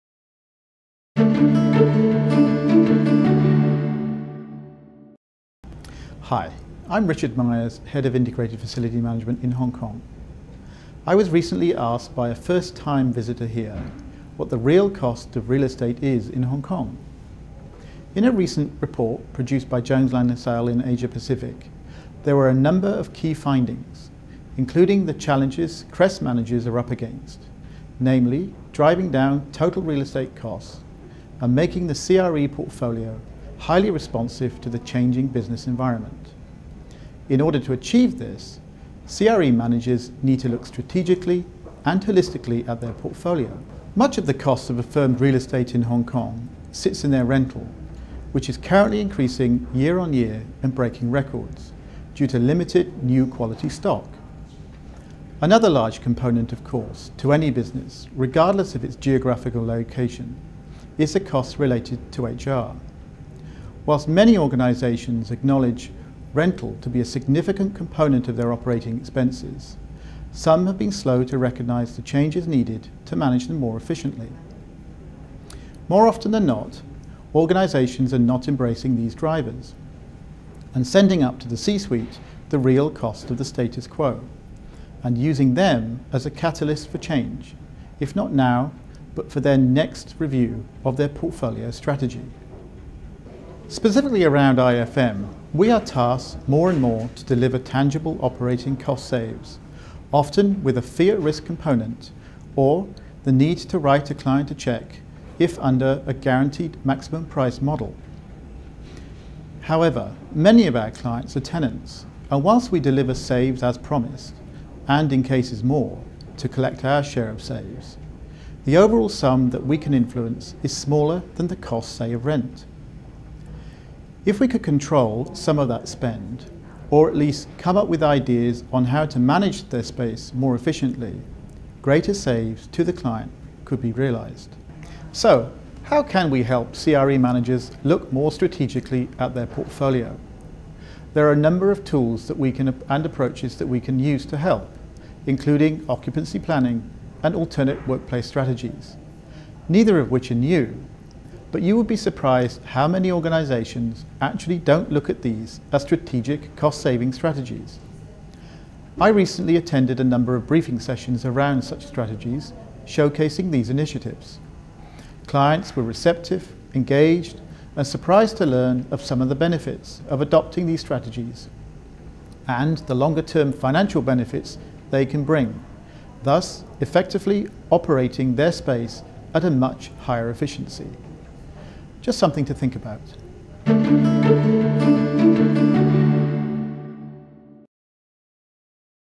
Hong Kong - Real cost of real estate
To drive down total real estate costs and make the CRE portfolio responsive to the changing business environment, CRE managers need to look strategically at their portfolio. Richard Myers, Head of Integrated Facilities Management for Jones Lang LaSalle, Hong Kong, shares his insights with you.